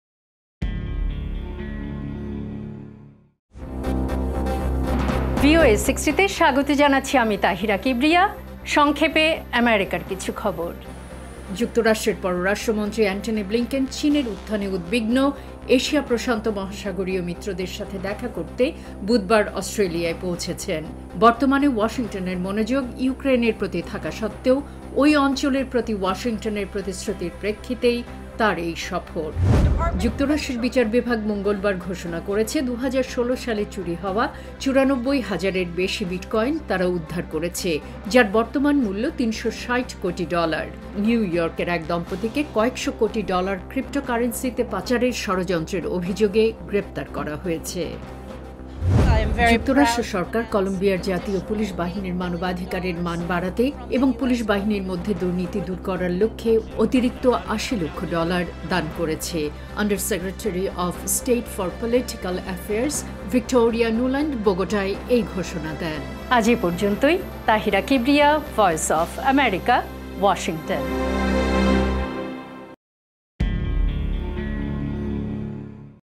আমেরিকার বিশেষ বিশেষ খবর ভিওএ 60-তে স্বাগত
যুক্তরাষ্ট্রের পররাষ্ট্রমন্ত্রী অ্যান্টনি ব্লিংকেন চীনের উত্থানে উদ্বিগ্ন এশিয়া-প্রশান্ত মহাসাগরীয় মিত্রদের সাথে দেখা করতে বুধবার অস্ট্রেলিয়ায় পৌঁছেছেন। বর্তমানে ওয়াশিংটনের মনোযোগ ইউক্রেনের প্রতি  থাকা সত্ত্বেও ঐ অঞ্চলের প্রতি আমেরিকার প্রতিশ্রুতির প্রেক্ষিতেই তার এই সফর।

যুক্তরাষ্ট্রের বিচার বিভাগ মঙ্গলবার ঘোষণা করেছে যে ২০১৬ সালে চুরি হওয়া ৯৪ হাজারের বেশি বিটকয়েন তারা উদ্ধার করেছে। যার বর্তমান মূল্য ৩৬০ কোটি ডলার। নিউ ইয়র্কের এক দম্পতিকে কয়েক’শ  কোটি ডলার ক্রিপ্টোকারেন্সিতে পাচারের ষড়যন্ত্রের অভিযোগে গ্রেপ্তার করা হয়েছে।

যুক্তরাষ্ট্র সরকার কলম্বিয়ার জাতীয় পুলিশ বাহিনীর মানবাধিকারের মান বাড়াতে এবং পুলিশ বাহিনীর মধ্যে দুর্নীতির দূর করার লক্ষ্যে অতিরিক্ত ৮০ লক্ষ ডলার দান করছে। আণ্ডার সেক্রেটারি অফ স্টেট ফর পলিটিক্যাল অ্যাফেয়ার্স ভিক্টোরিয়া নুল্যান্ড বোগোটায় এই ঘোষণা দেন।